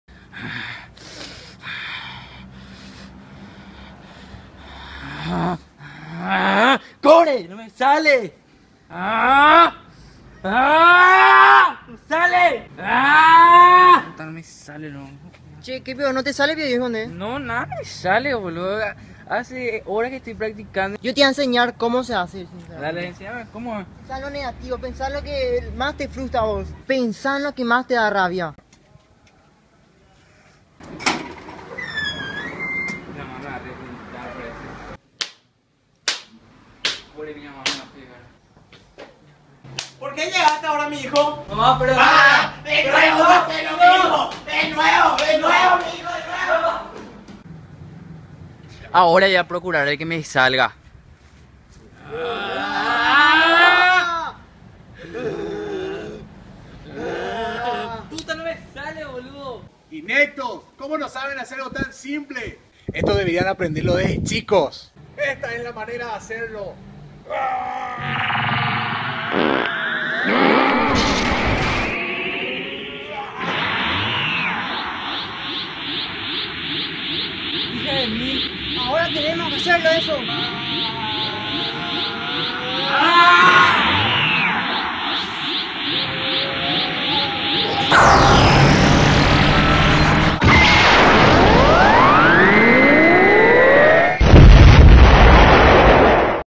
Kame hame ha paraguayo
Te presentamos este pequeño video donde parodiamos un Kame Hame Ha! Compartilo!